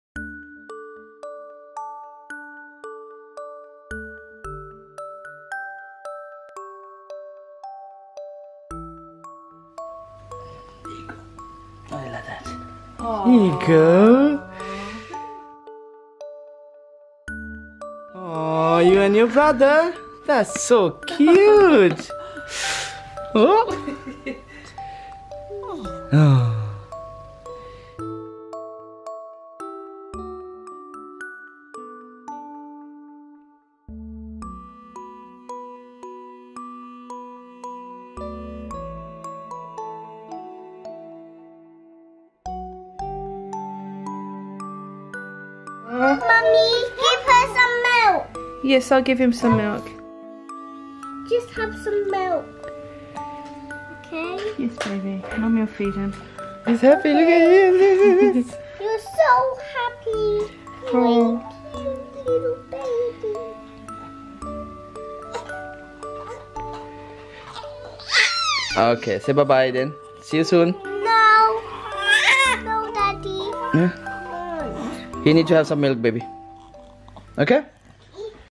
SO MUCH LOVE! Sister holding her NEWBORN Baby Brother for the FIRST TIME! Cute Baby Shorts
Rochelle holds her newborn baby brother for the first time in this short clip. She gives so much love and affection to her baby brother Leo that she does not want to let him go when it is time for his milk.

- cute babies shorts
- so much love! Sister holding her newborn baby brother for the first time